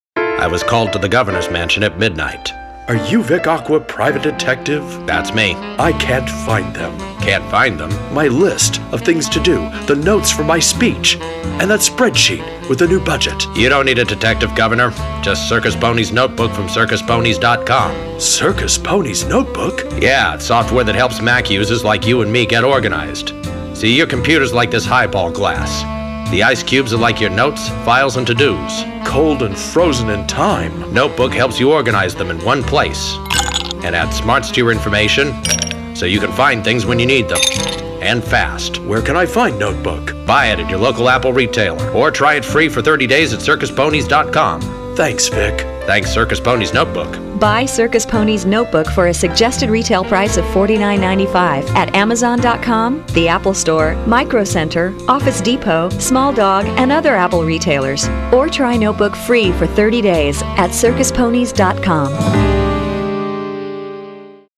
Circus Ponies NoteBook Funny Radio Ad - Governor's Mansion
Vic Aqua helps the Governor find his to do list, the notes from his speech, and the spreadsheet with the new budget.
The detective tells him to get Circus Ponies Notebook from circusponies.com, a software that helps Mac users get organized.
A computer is like a highball glass, the ice cubes are like files, notes and to dos. Cold and frozen in time. Circus Ponies NoteBook helps organize them in one place and adds smarts to them, so that the information can be found when needed and fast.
Circus Ponies Notebook can be found at the local Apple retailer or at circusponies.com